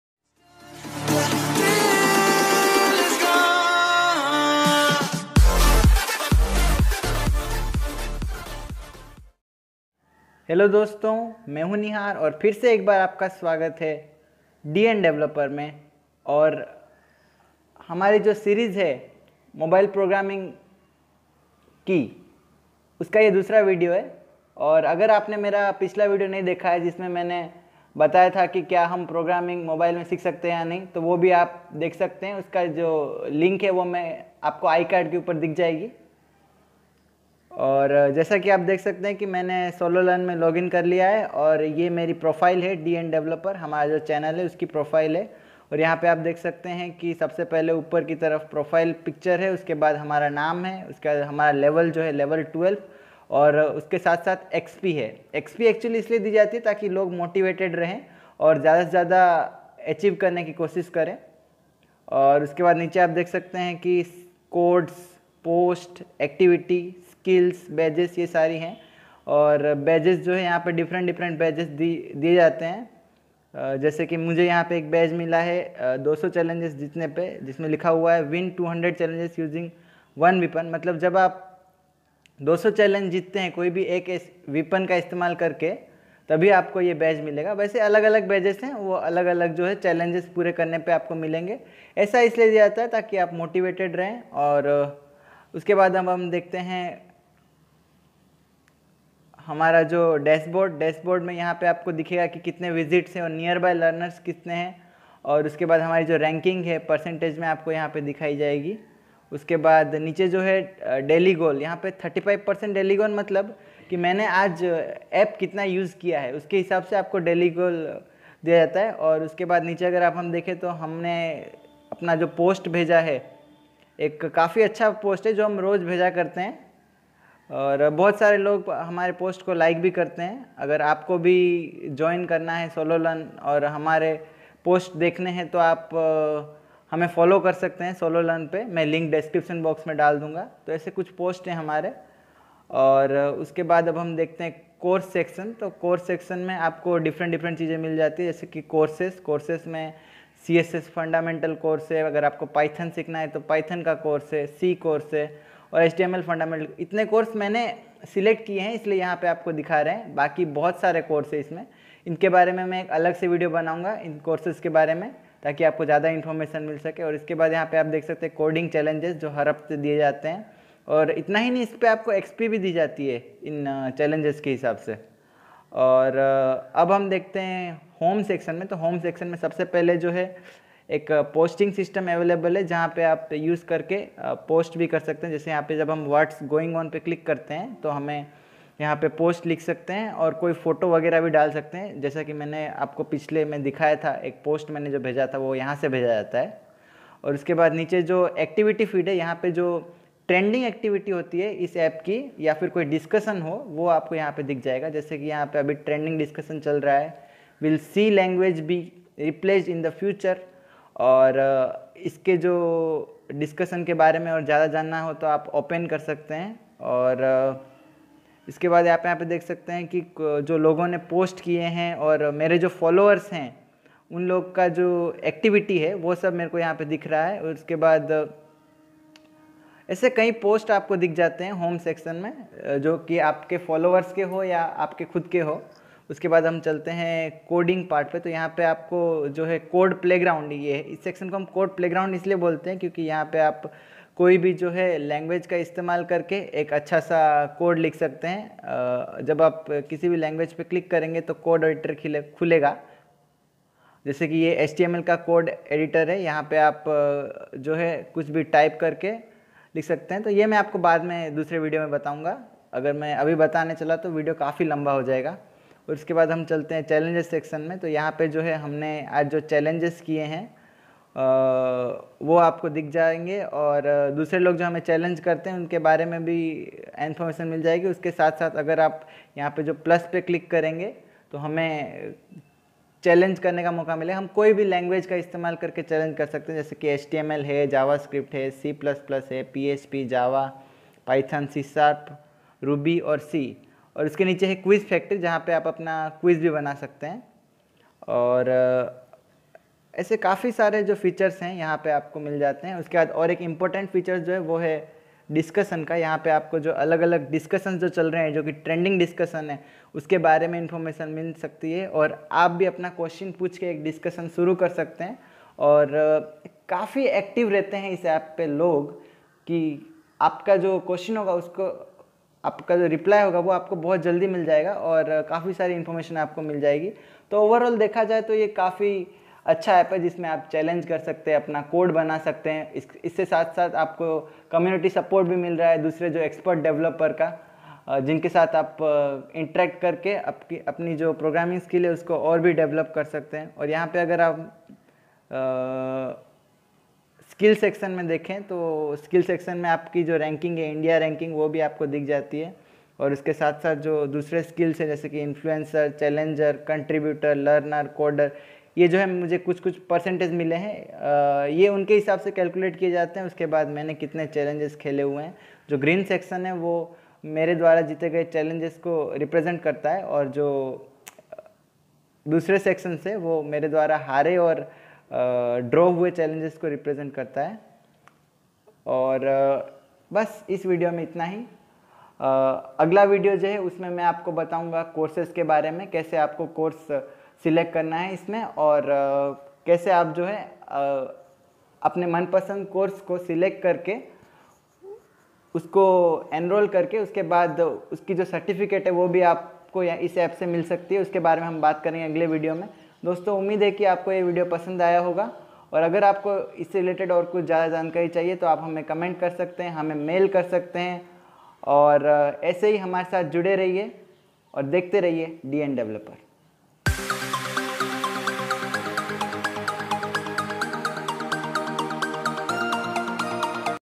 Sololearn Android App Review | Best Programming App For Mobile| Mobile Programming Series #2
It is the second video of mobile programming series.This video contains a short review of the best mobile programming app sololearn.
So why waiting watch this amazing video and know about the features of the solo learn app and begin your coding carrer.
Follow us on sololearn:
Link to our first video of mobile programming series:
Can we learn programming on mobile??

COMMENT:If you want to know more and have some suggestions...
Happy Coding...☺☺☺